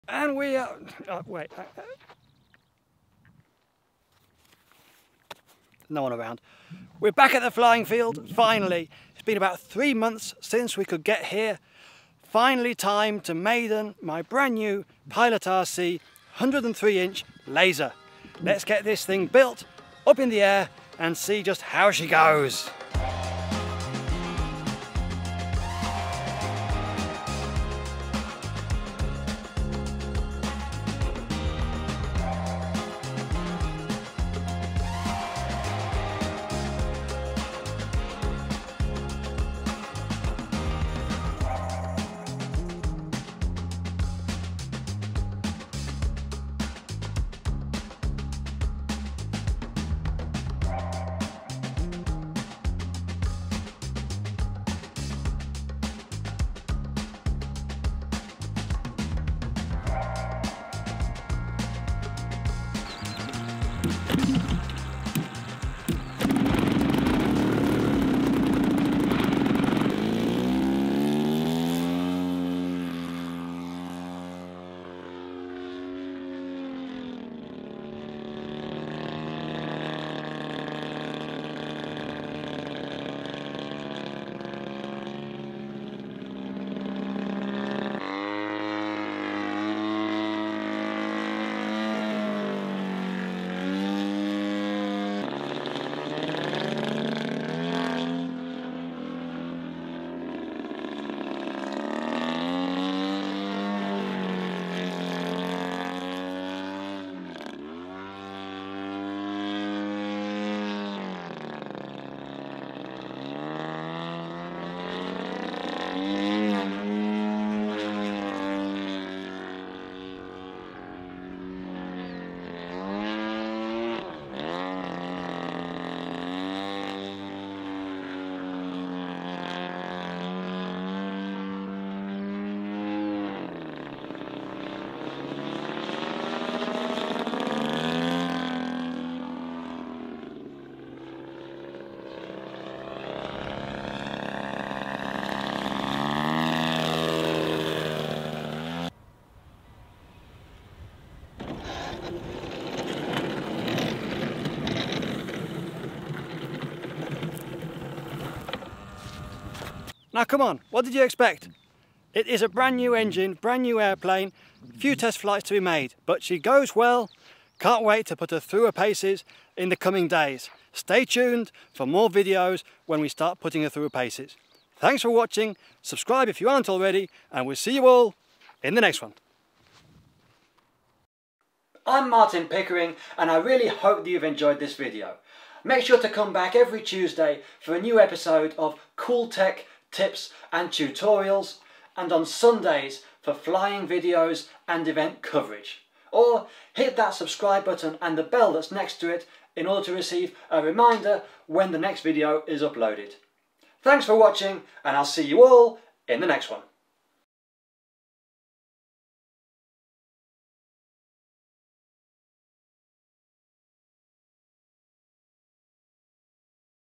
NEW LASER! MAIDEN and FIRST FLIGHT after quarantine!
Feels great to be back at the field! Never have I had to wait so long to get a first flight on a plane after finishing the build!

Martin Pickering